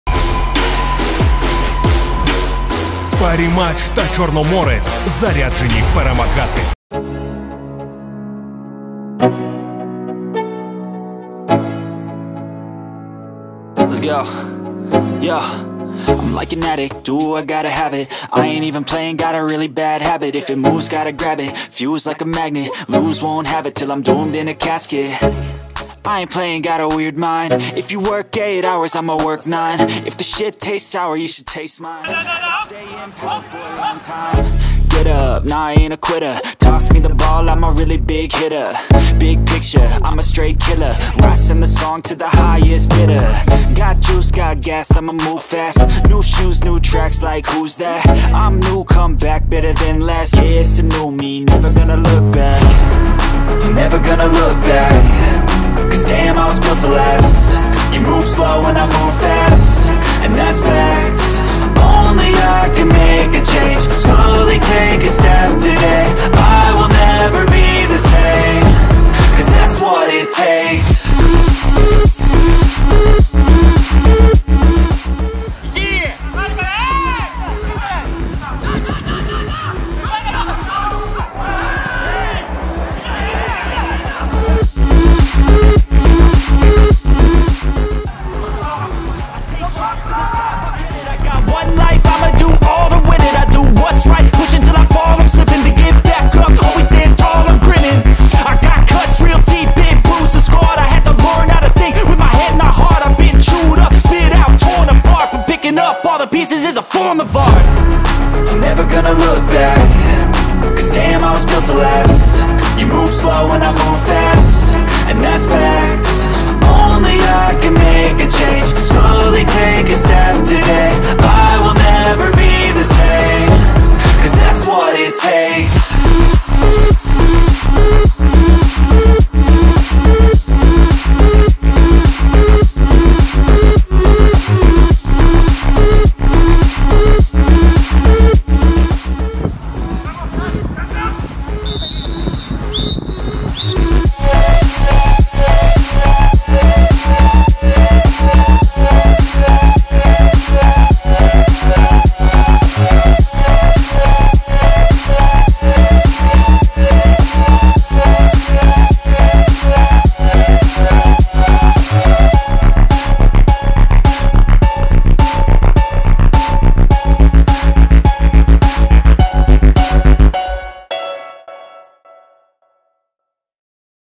ЧОденник зборів у Туреччині. День 9
Поздравляем с первой победой💥

Две прекрасные новости сегодняшнего дня: первая победа обновлённой команды Романа Григорчука и наши вратари не пропустили ни одного мяча в ворота «Черноморца».

Вторая игра выглядела подинамичнее. Как результат - 2 гола в ворота «Lechia Gdańsk». Хотя могло быть и больше: у Ваната и Кадири была возможность, как минимум, удвоить счёт, но польский вратарь был неплох (ну это так, чтоб не сильно хвалить игроков команды-соперника😬)

«Lechia Gdańsk» занимает 5-е место в Чемпионате Польши на данный момент. В последнее время показывает хороший прогресс в своей игре, что доказывают выступление в еврокубках и победа в Кубке Польши в 2019 году.

Тем ценнее сегодняшняя победа «Черноморца»🖤💙